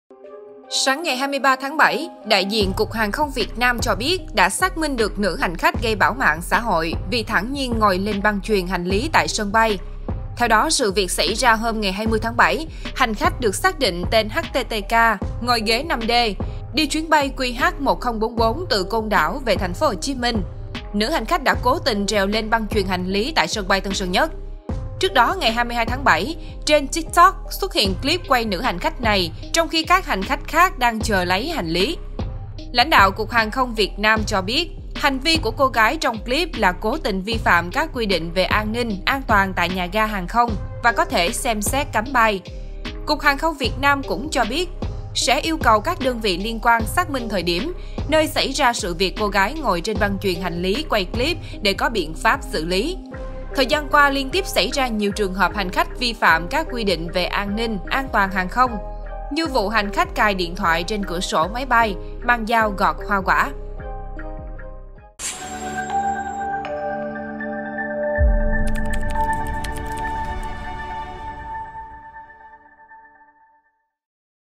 Cô gái ngồi trên băng chuyền hành lý tại sân bay Tân Sơn Nhất là cố tình, có thể xem xét cấm bay
Sáng 23-7, lãnh đạo Cục Hàng không Việt Nam cho biết, hành vi của cô gái trong clip là cố tình vi phạm các quy định về an ninh, an toàn tại nhà ga hàng không và có thể xem xét cấm bay.